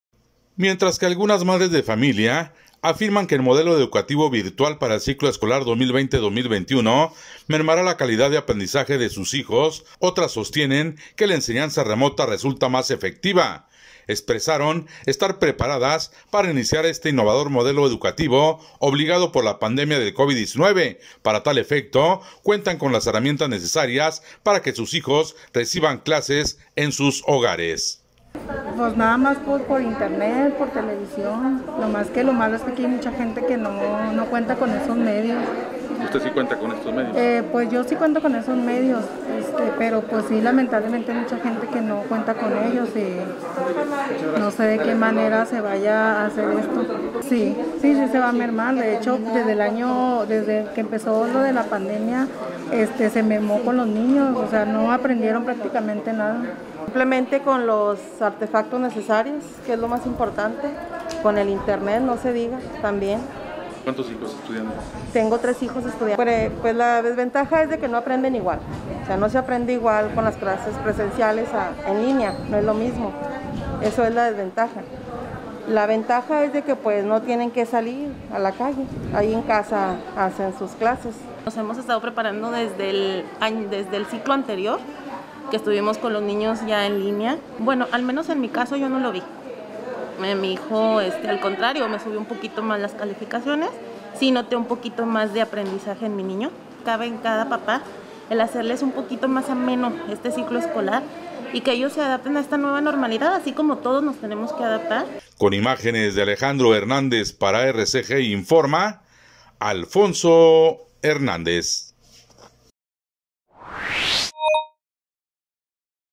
Se preparan en casa para Ciclo Escolar En Línea
Madres de familia expresaron estar preparadas para iniciar este innovador modelo educativo obligado por la pandemia del Covid-19